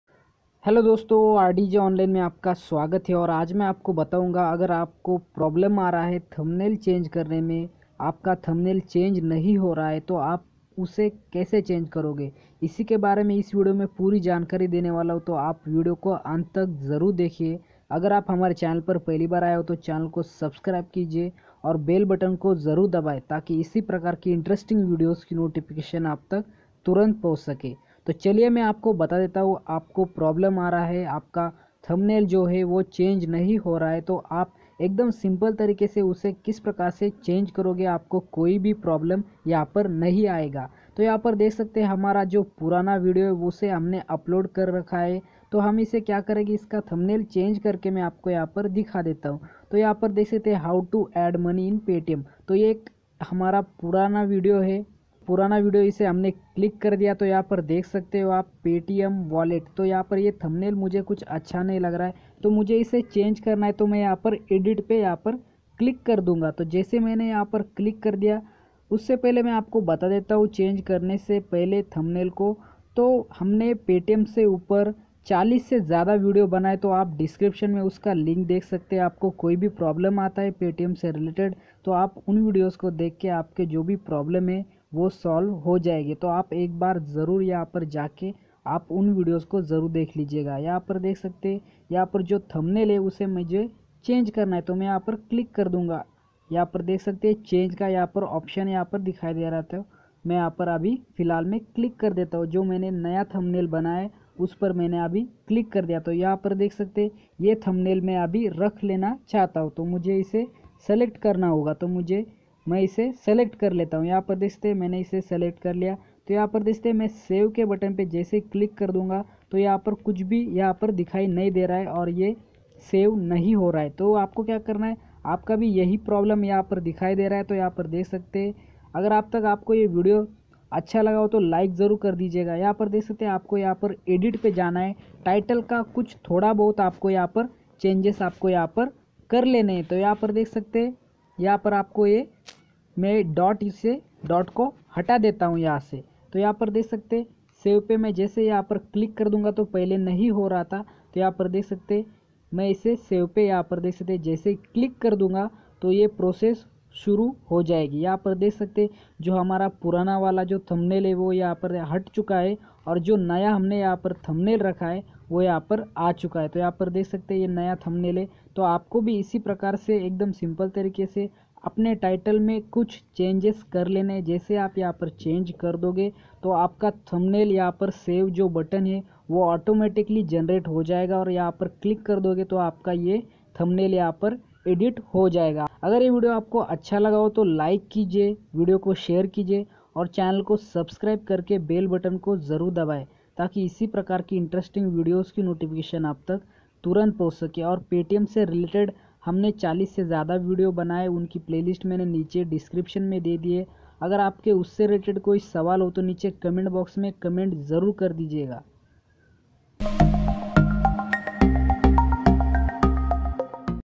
थंबनेल कैसे लगाएं। Change Thumbnail YouTube। How To Change Thumbnail Kaise Badale
थंबनेल कैसे लगाएं। Change Thumbnail YouTube।
How To Change Thumbnail Kaise Badale.

ABOUT VIDEO:-
Is me aapko youtube thumbnail kaise badalte hai agar aapko thumbnail change karna hai to change thumbnail kar sakte ho. Thumbnail change karne me problem aa rahi hai to aap mobile phone se youtube ke thumbnail ko ytsudio se kaise change karoge, kaise badli he, phutane video old video ka thumbnail change kase kare new thumbnail add kaise kare yah is video mai bataya hai.

This Video Just Only Information Poropse